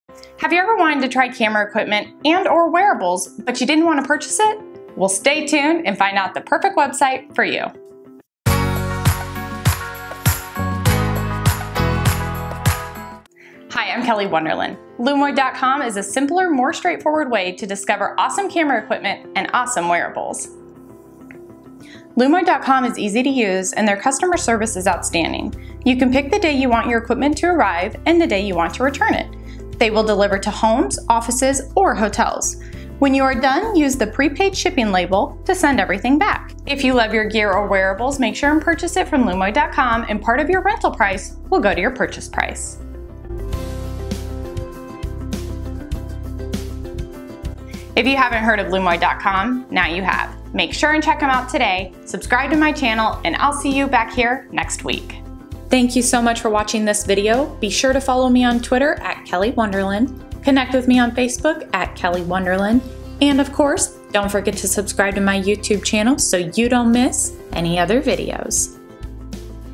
LUMOID: RENT BUY OR TRY
Lumoid.com lets you rent wearables or camera gear before purchasing. CLICK TO SEE MORE ↓

* Make sure and give this video a thumbs up*